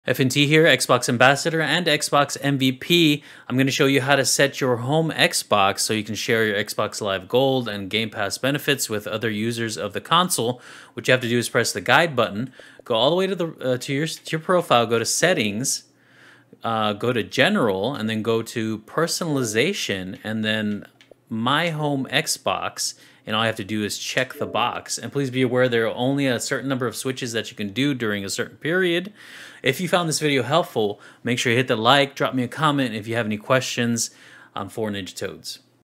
How to set your home Xbox | How to game share
In this short video I show you how to set you home Xbox for game sharing with other console users.  You are only allowed a few switches a year so don't over use it.

Become a Paid Member today!